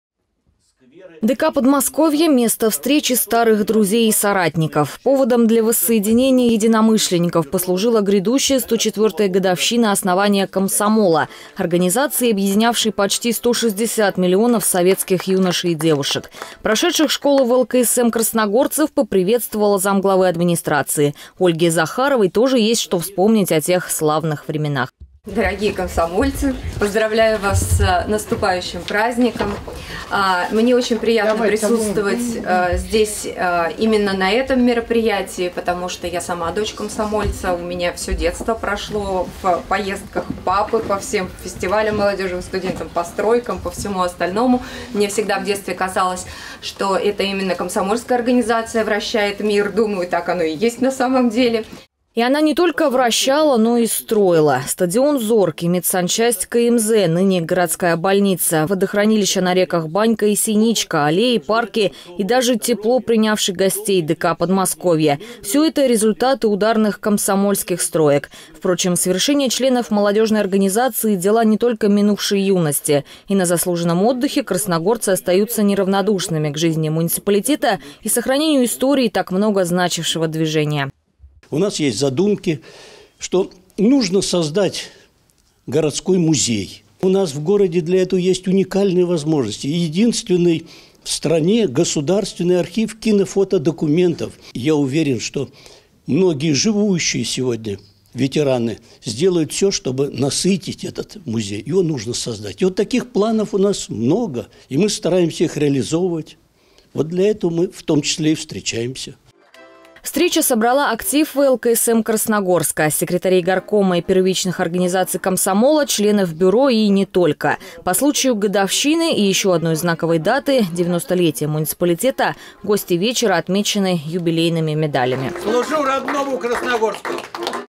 КРТВ. «Запишите меня в комсомол»
104-ю годовщину основания ВЛКСМ, или комсомола, отпраздновали в Красногорске. На торжественном вечере в ДК «Подмосковье» чествовали активистов движения.